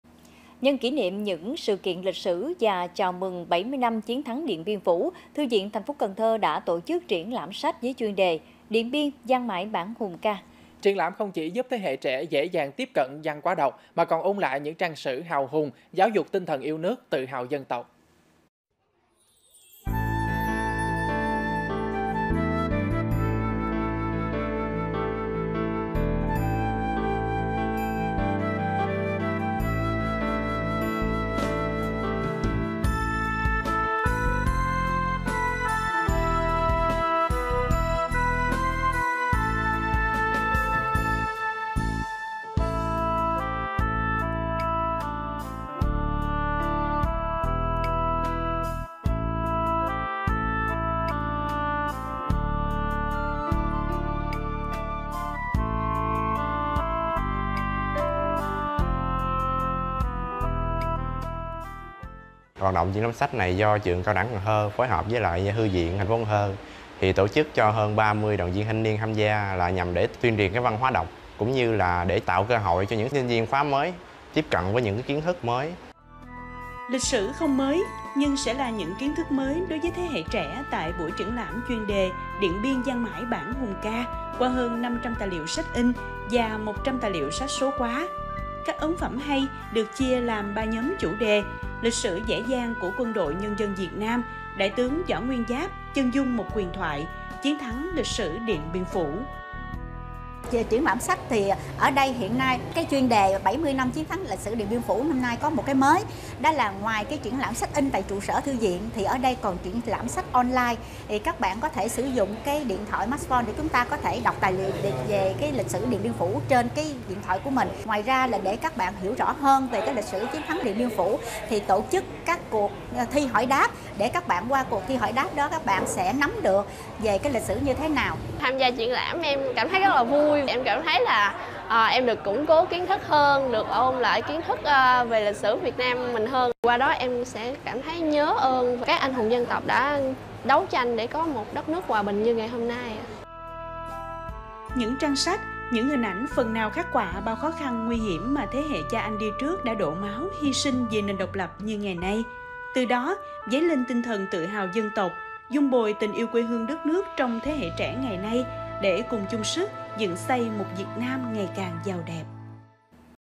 Giáo dục lịch sử qua những trang sách | Cần Thơ TV
Nhân kỷ niệm những ngày 30/4 lịch sử và chào mừng 70 năm chiến thắng Điện Biên Phủ, Thư viện TP. Cần Thơ đã tổ chức triển lãm sách với chuyên đề “Điện Biên vang mãi bản hùng ca”.